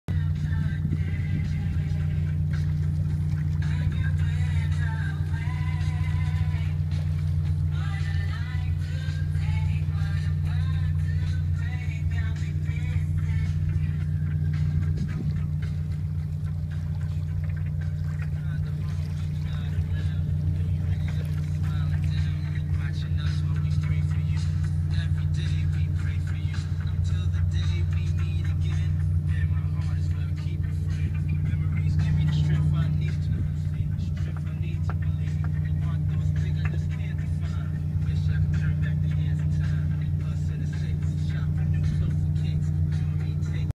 Bluefin tuna off Devon! (by Alan Steer)
Huge bluefin tuna swims around fishing boat off Devon today